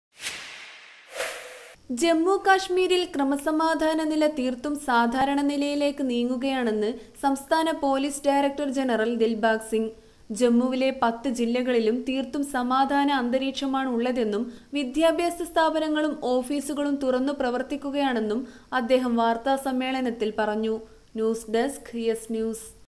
ജമ്മു കാശ്മീരില്‍ ക്രമസമാധാന നില തീര്‍ത്തും സാധാരണ നിലയിലേക്ക്
ജമ്മു കാശ്മീരില്‍ ക്രമസമാധാന നില തീര്‍ത്തും സാധാരണ നിലയിലേക്ക് നീങ്ങുകയാണെന്ന്  സംസ്ഥാന പോലീസ് ഡയറക്ടര്‍ ജനറല്‍ ദില്‍ബാഗ്‌സിംഗ്